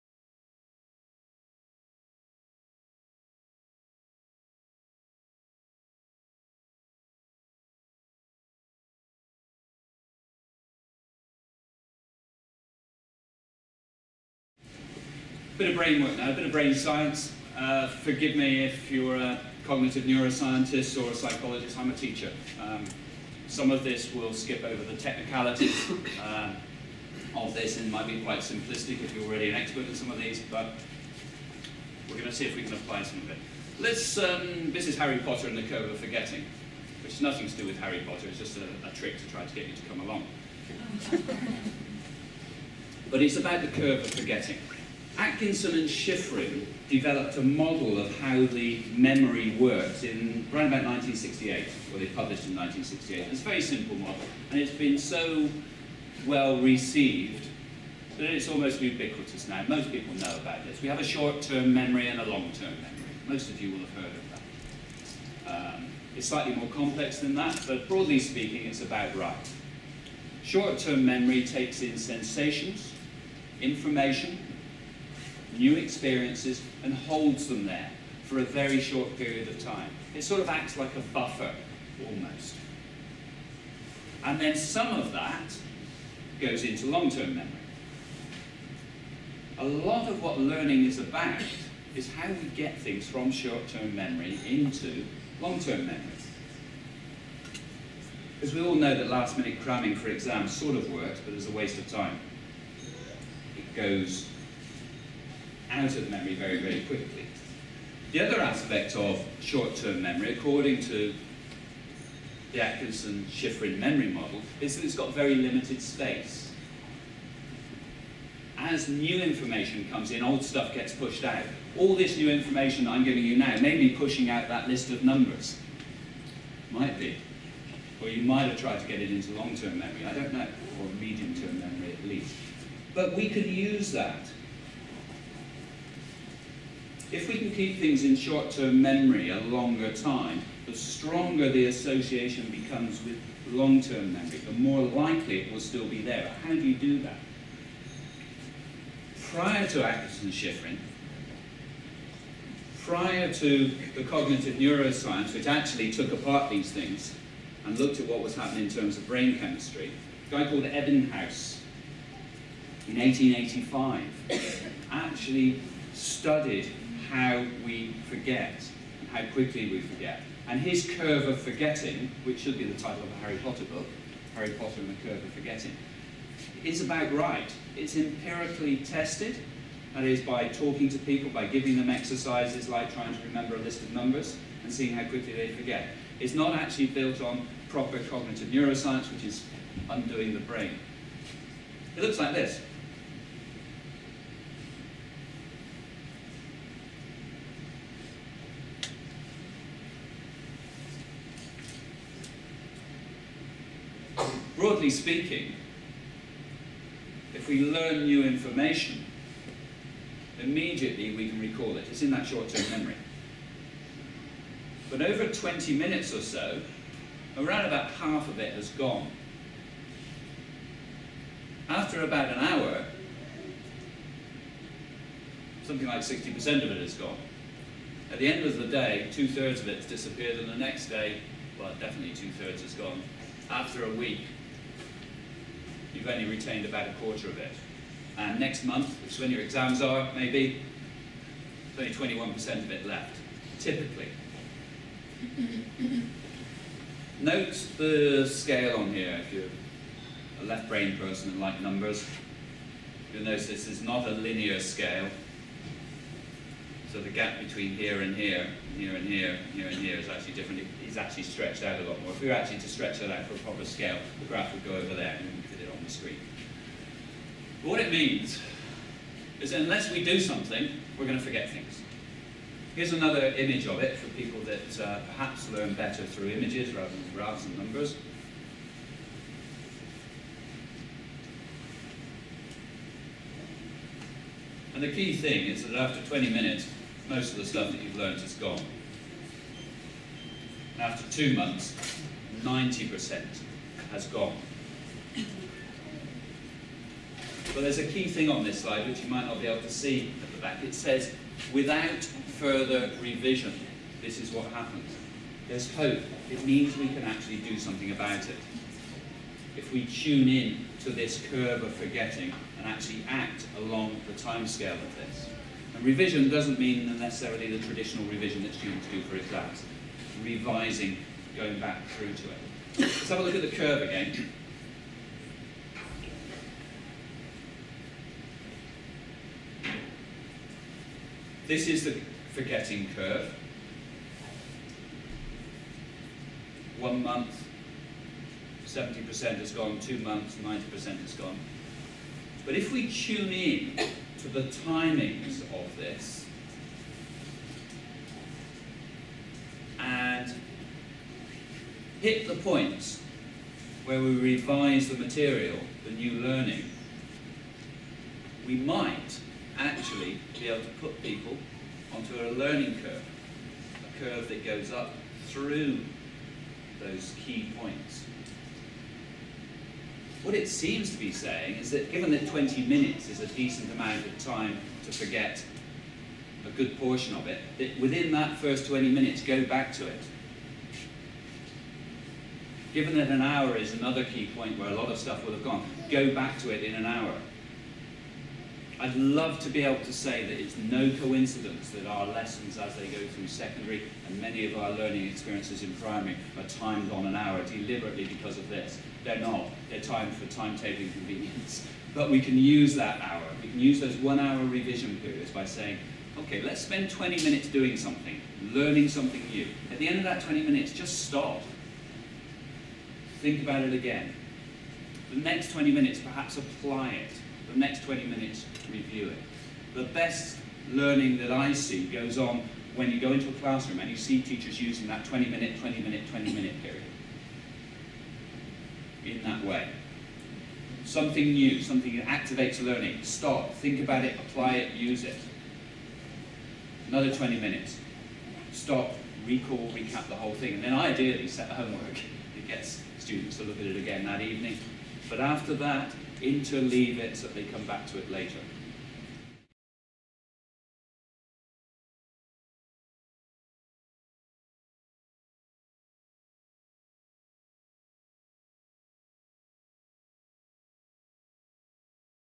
Desirable Difficulties – tips to turbocharge learning - Part 1
Principal Richard Dyer talking about Harry Potter and the Curve of Forgetting.